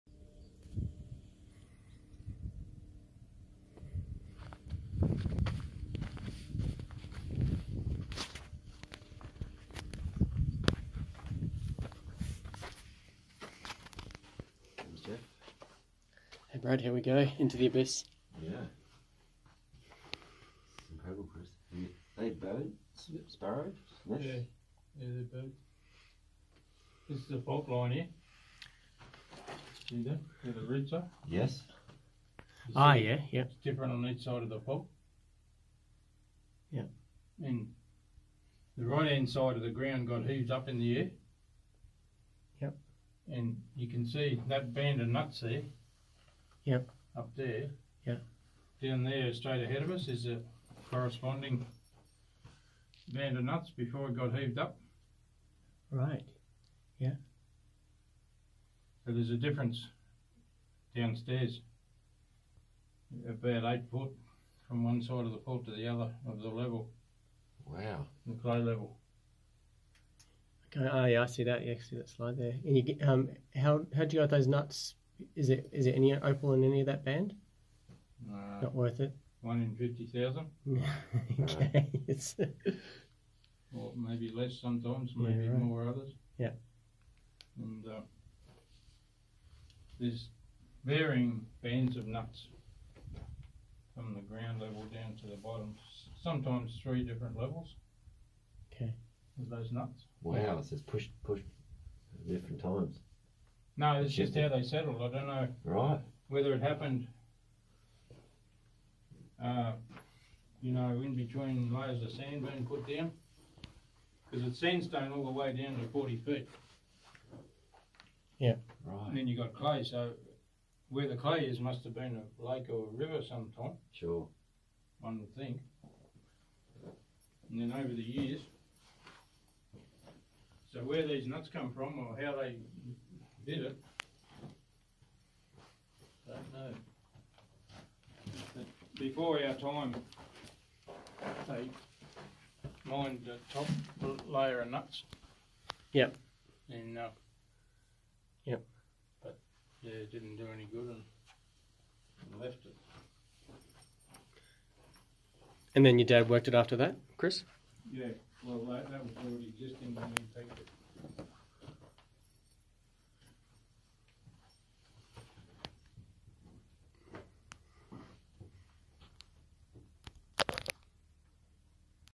Heading down underground - Opal Mine in Yowah
Exclusive access down an incredible mine.  Sorry this mine is not available to the general public.